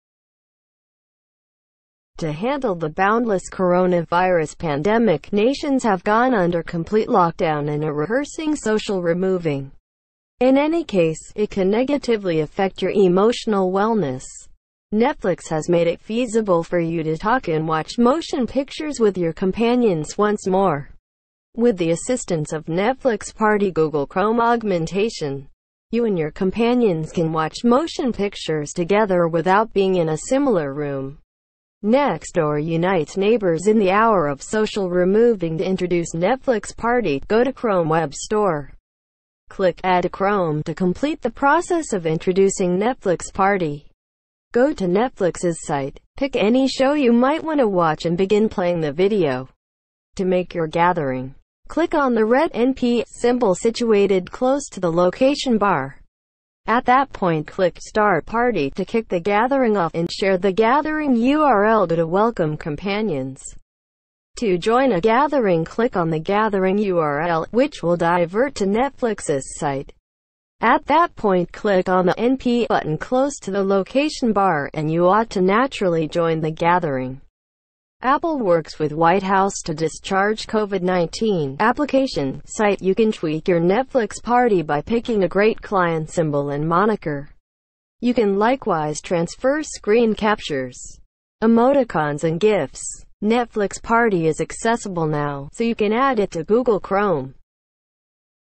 How to use netflix party.
Netflix Party: Now you can watch movies together under coronavirus lockdown